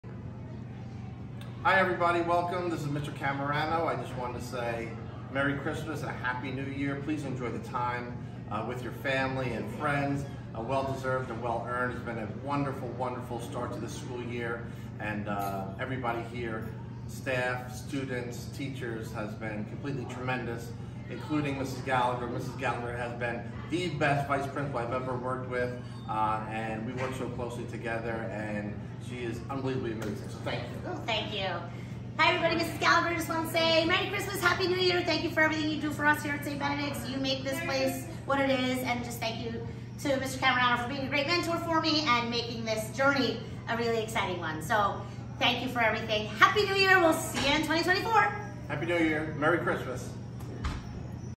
Principal’s Weekly Update 12.21.23
Mr. Cammarano talks about the happenings this past week and what’s coming up in the week ahead.